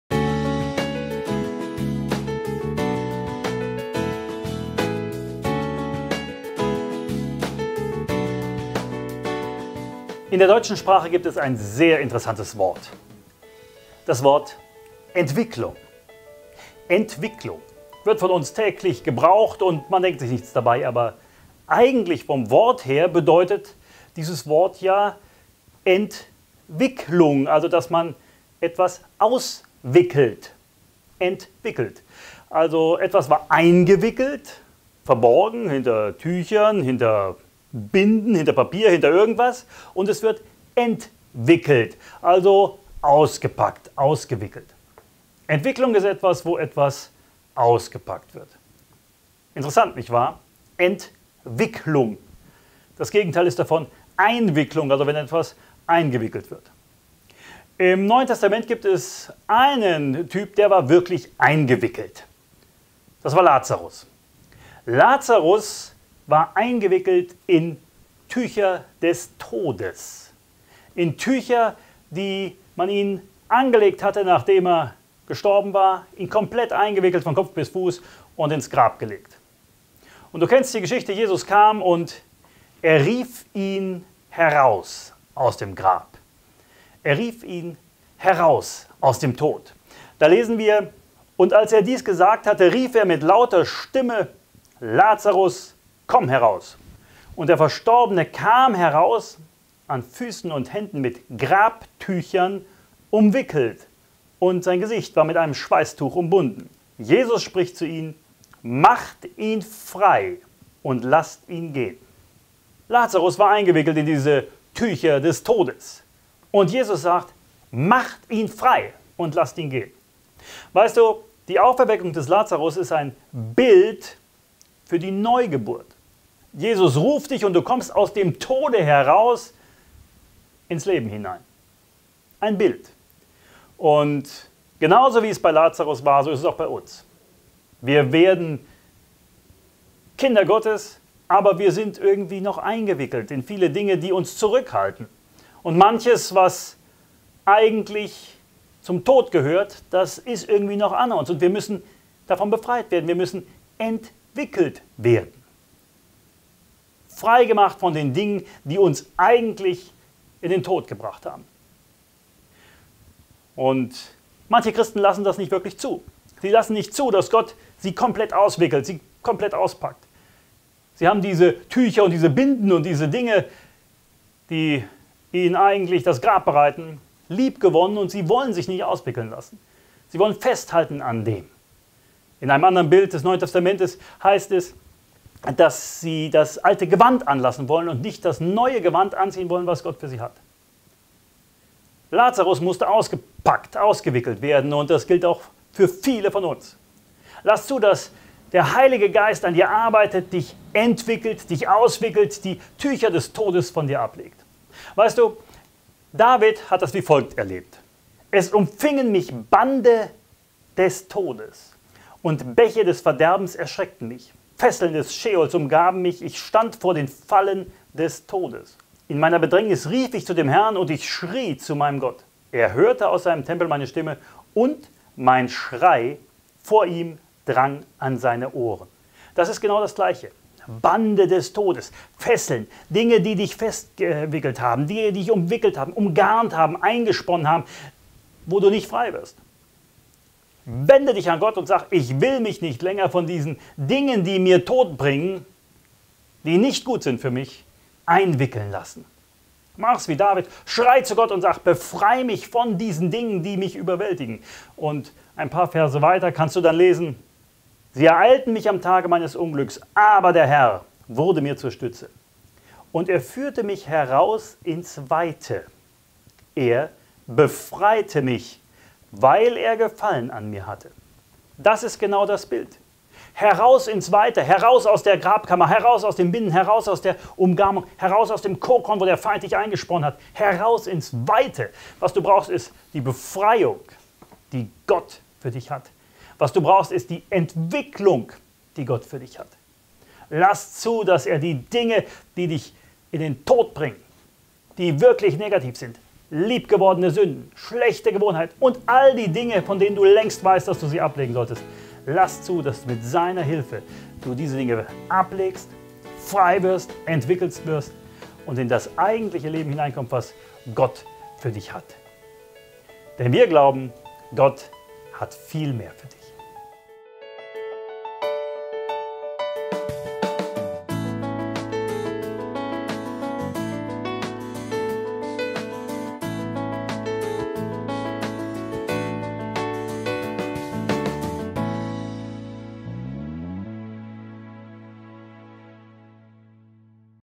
Ent-Wicklung
Die deutsche Sprache hat ein interessantes Wort parat: Entwicklung. Eigentlich bedeutet es vom Wort her: Wenn man etwas ent-wickelt, dann wickelt man es aus, man befreit es von irgendetwas, in das es eingewickelt ist. Es ist verborgen hinter Stoff, Binden, Papier oder was auch immer und man ent-wickelt es. In der Bibel finden wir jemanden, der echt ent-wickelt wurde: Lazarus.

Er war in seine Leichentücher – Tücher des Todes - gewickelt und als Jesus ihn aus dem Grabe herausrief, kam er ziemlich eingewickelt heraus. Er musste erst einmal von den Tüchern befreit werden.

Wenn der Feind versucht, dich mit Tüchern und Binden und Schlingen des Todes einzuwickeln, wenn er dich umgarnt und einspinnt, dann wende dich Gott zu und sage: Herr, entwickle mich, befreie mich. So tat es auch David. Er sagte zu Gott in genau einer solchen Situation, in der sich völlig überwältigt fühlte: Herr, hol mich hier raus! Und Gott tat es.

Es umfingen mich Bande des Todes, und Bäche des Verderbens erschreckten mich. Fesseln des Scheols umgaben mich, ich stand vor den Fallen des Todes. In meiner Bedrängnis rief ich zum HERRN, und ich schrie zu meinem Gott. Er hörte aus seinem Tempel meine Stimme, und mein Schrei vor ihm drang an seine Ohren.
Psalm 18,5-7

Sie ereilten mich am Tage meines Unglücks, aber der HERR wurde mir zur Stütze. Und er führte mich heraus ins Weite, er befreite mich, weil er Gefallen an mir hatte.
Psalm 18,19-20

Wo ist, Tod, dein Sieg? Wo ist, Tod, dein Stachel? Der Stachel des Todes aber ist die Sünde, die Kraft der Sünde aber das Gesetz. Gott aber sei Dank, der uns den Sieg gibt durch unseren Herrn Jesus Christus!
1.Korinther 15,55-57

Er hat die Gewalten und die Mächte völlig entwaffnet und sie öffentlich zur Schau gestellt. In ihm hat er den Triumph über sie gehalten.
Kolosser 2,15

Der hat uns gerettet und berufen mit heiligem Ruf, nicht nach unseren Werken, sondern nach seinem eigenen Vorsatz und der Gnade, die uns in Christus Jesus vor ewigen Zeiten gegeben, jetzt aber offenbart worden ist durch die Erscheinung unseres Retters Christus Jesus, der den Tod zunichte gemacht, aber Leben und Unvergänglichkeit ans Licht gebracht hat durch das Evangelium,
2.Timotheus 1,9-10

Und als er dies gesagt hatte, rief er mit lauter Stimme: Lazarus, komm heraus! Und der Verstorbene kam heraus, an Füßen und Händen mit Grabtüchern umwickelt, und sein Gesicht war mit einem Schweißtuch umbunden. Jesus spricht zu ihnen: Macht ihn frei und lasst ihn gehen!
Johannes 11,43-44

Da kommt Simon Petrus, der ihm folgte, und ging hinein in die Gruft und sieht die Leinentücher daliegen und das Schweißtuch, das auf seinem Haupt war, nicht zwischen den Leinentüchern liegen, sondern für sich zusammengewickelt an einem besonderen Ort.
Johannes 20,6-7